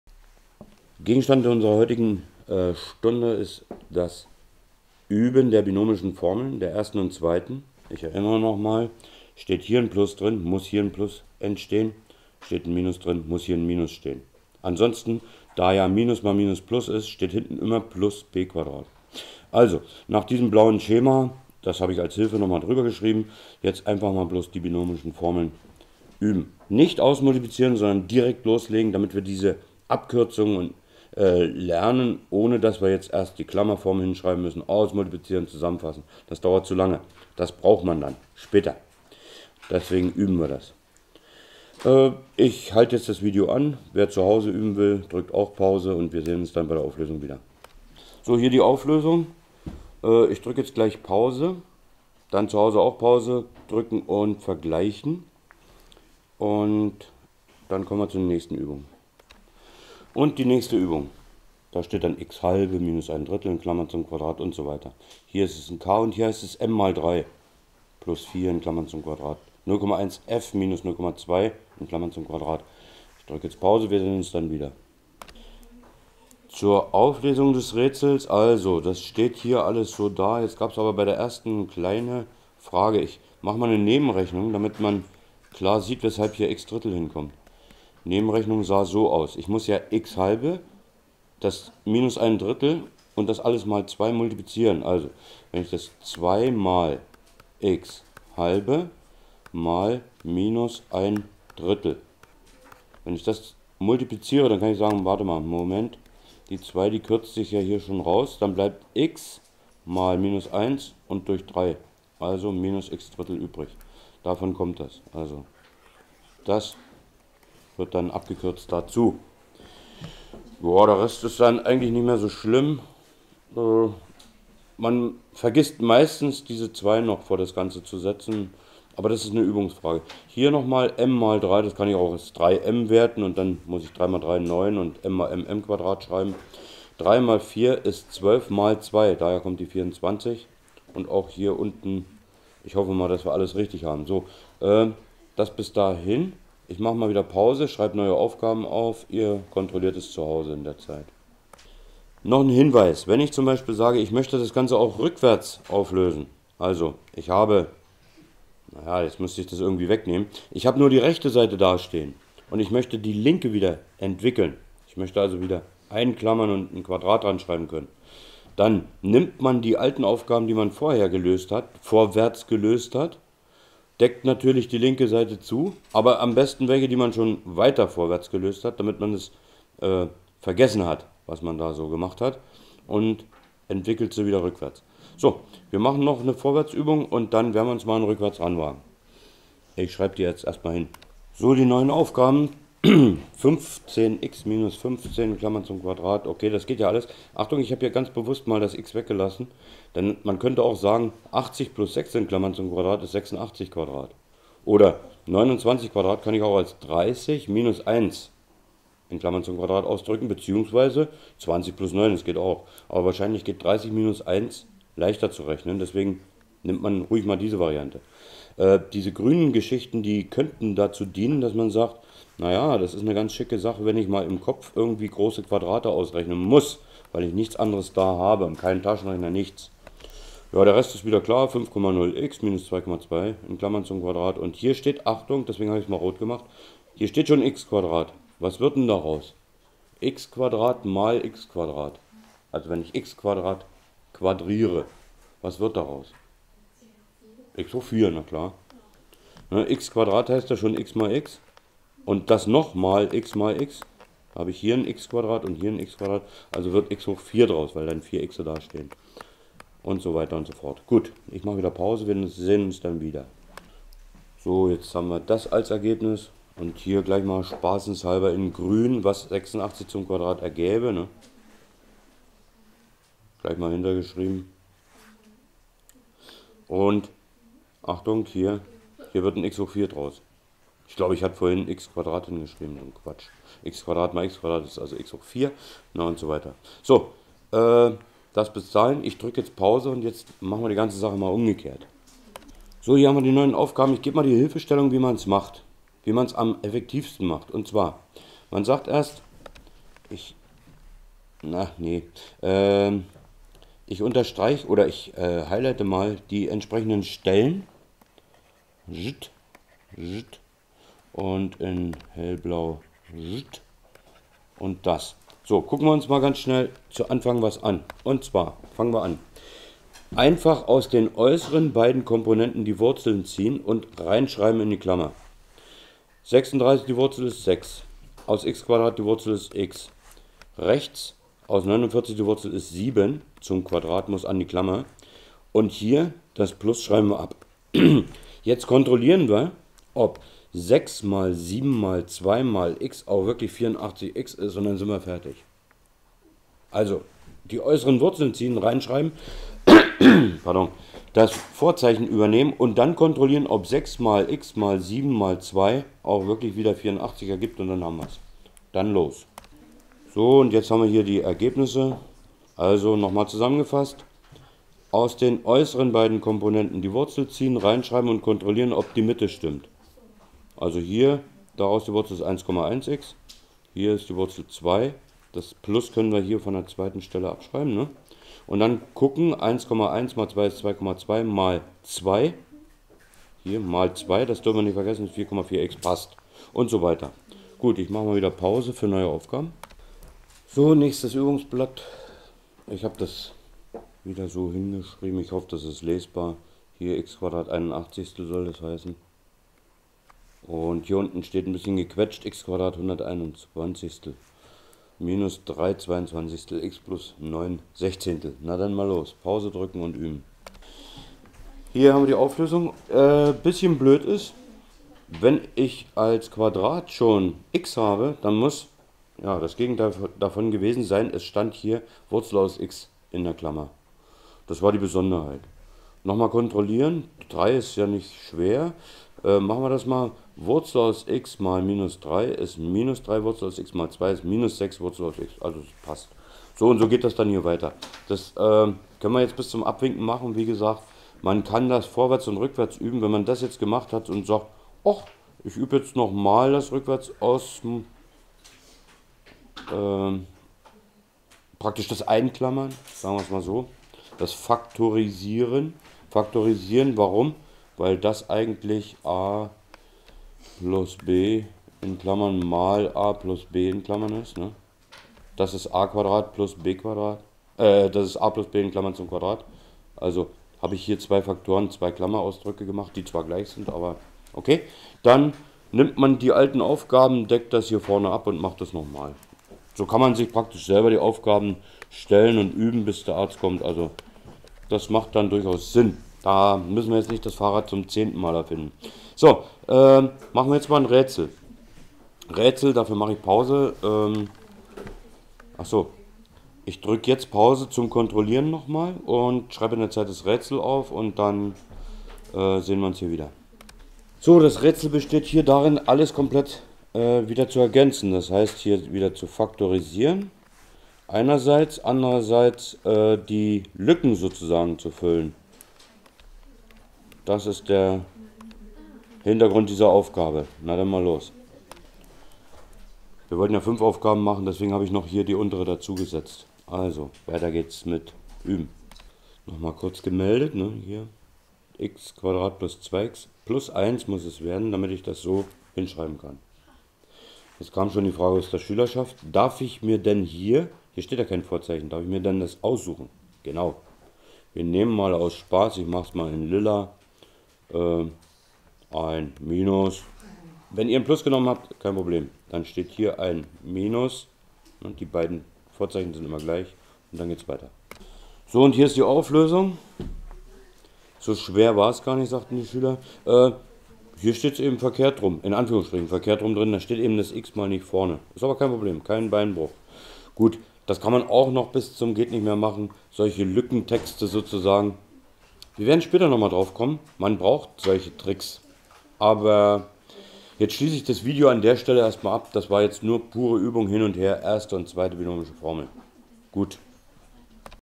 Binomische Formeln rückwärts +vorwärts rechnen üben, +Brüche +Dezimalzahlen, BiFo01
Bitte, helfen Sie uns Endlich wieder (Sapere aude!) Dezimalzahlen und Brüche, sowie deren Verarbeitung gehören einfach dazu.
Binomische Formeln sollte man im Schlaf können, weil sie vieles erleichtern (zB. das Ausmultiplizieren von Klammerausdrücken).
Auch erlauben sie die Faktorisierung von Termen, also die Umformung von bestimmten Summen und Differenzen in Produkte, was bei der Vereinfachung von Bruchtermen, beim Radizieren von Wurzeltermen sowie Logarithmenausdrücken sehr oft die einzige Lösungsstrategie darstellt.
Eigentlich sie Spezialfälle des Distributivgesetzes für algebraische Summen (jedes Glied der einen wird mit jedem der anderen Summe multipliziert). (Quelle: Wikipedia) Aufgaben mit Lösungen zur Wiederholung und Übung für zu Hause.

08 2015_01_14 :zzzzz_intern